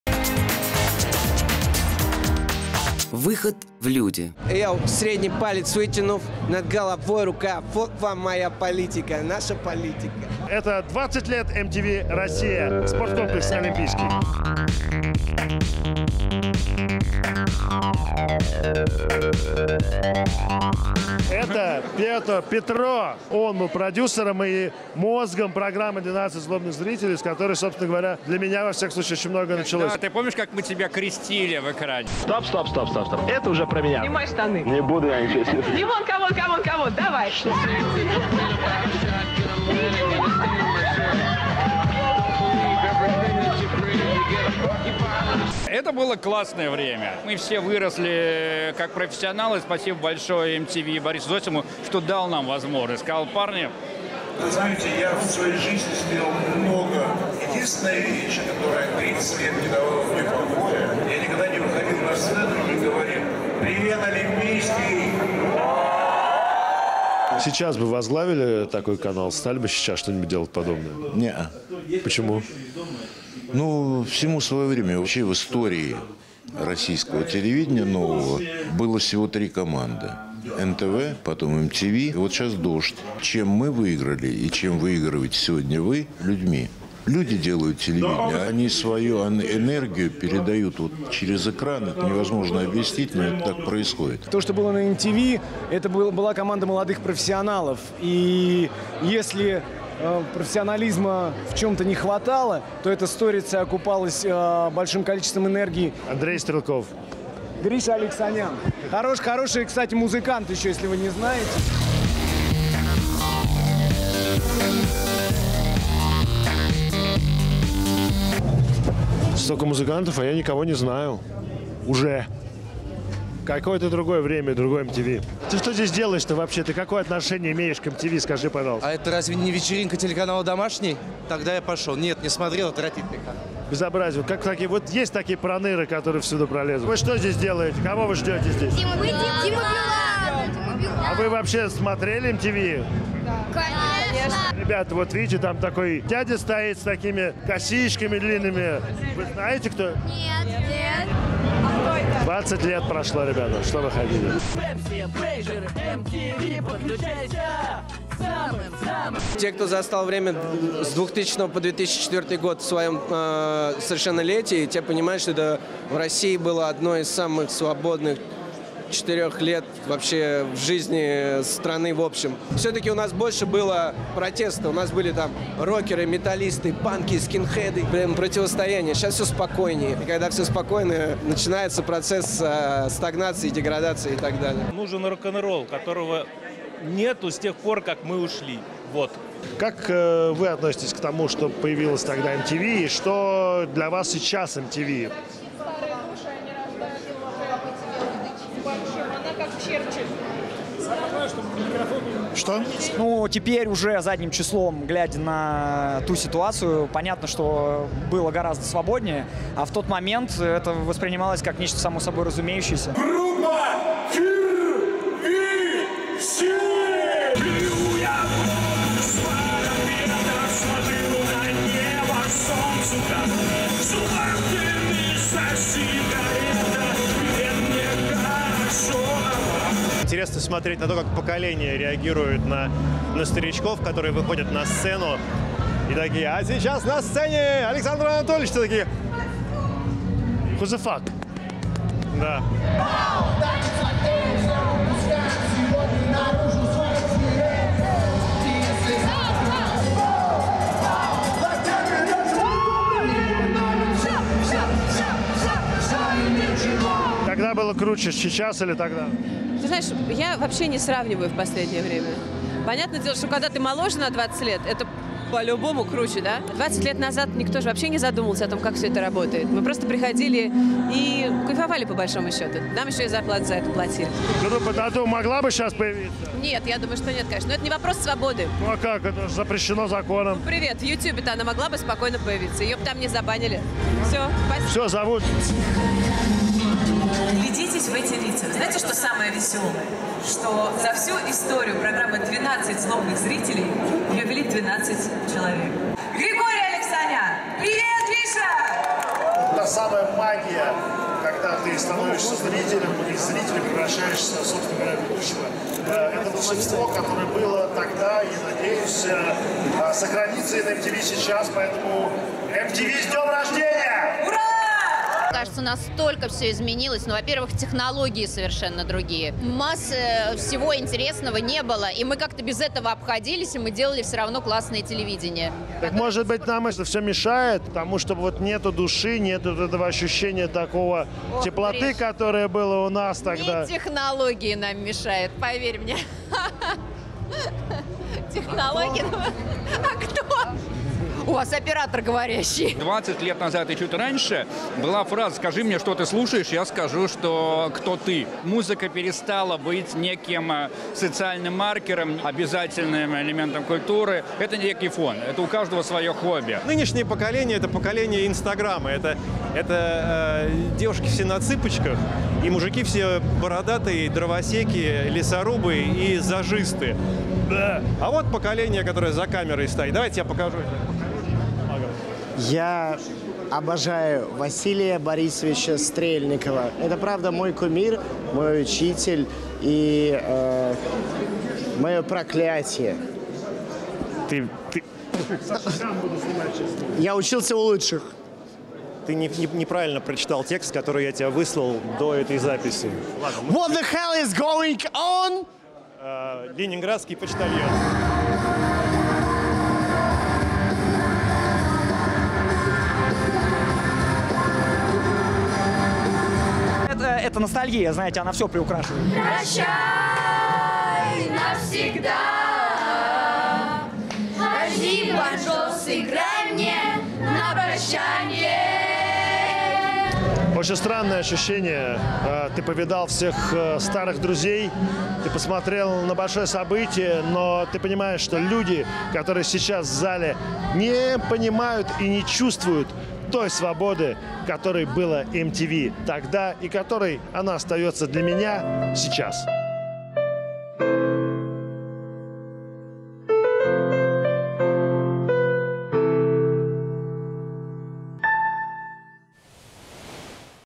«Это были самые свободные четыре года в жизни России»: 20-летие MTV Russia в «Олимпийском»
27 сентября канал MTV Russia отметил свой 20-й юбилей концертом в «Олимпийском». Григорий Алексанян побывал на концерте, поговорил со «старожилами» MTV и отметил круглую дату вместе с ними.